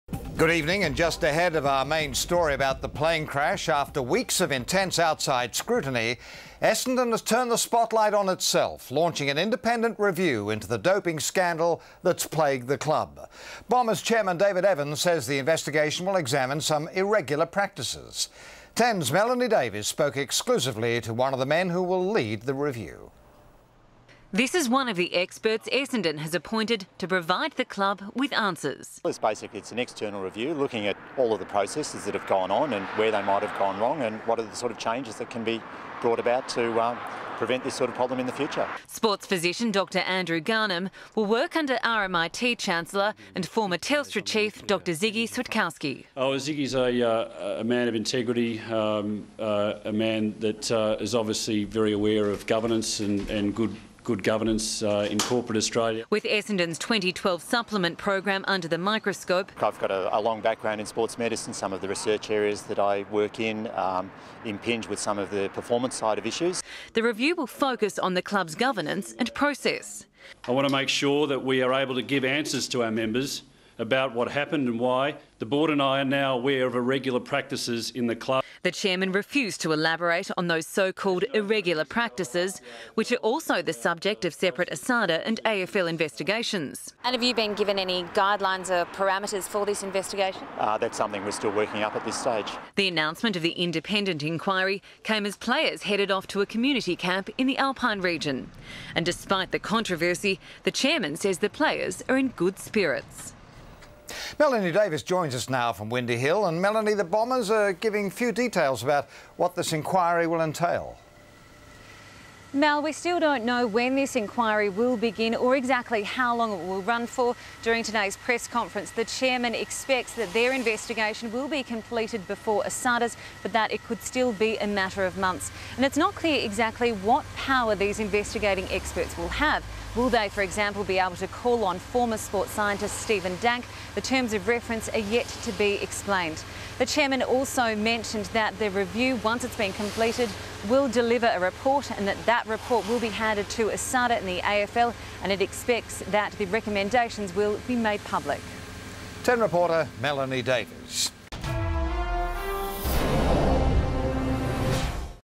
Crisis Call
After weeks of intense outside scrutiny, Essendon has turned the spotlight on itself, launching an independent review into the doping scandal that's plagued the club.